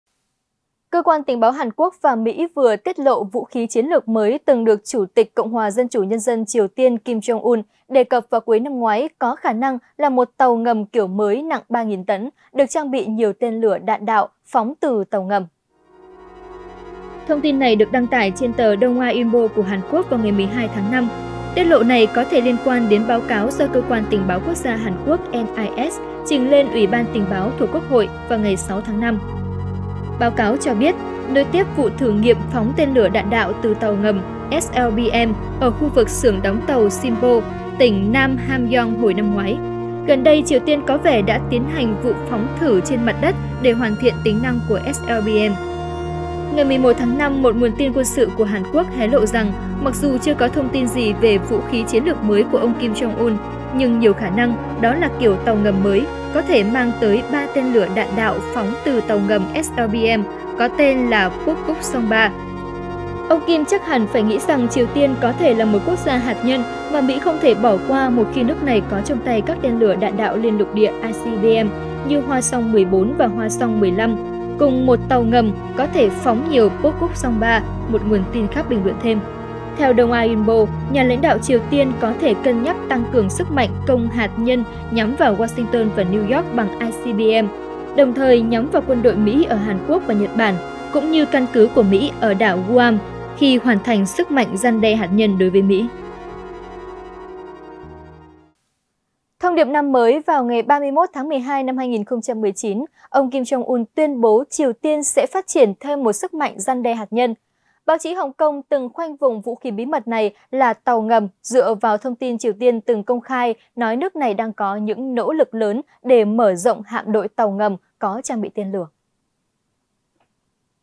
Tin tức MỚI NHẤT: Hé lộ 'vũ khí chiến lược mới' của Kim Jong Un
Tin Tức Mới Nhất:"Vũ khí chiến lược mới" mà nhà lãnh đạo họ Kim nhắc đến trong bài phát biểu cuối năm ngoái nhiều khả năng là con tàu ngầm 3.000 tấn.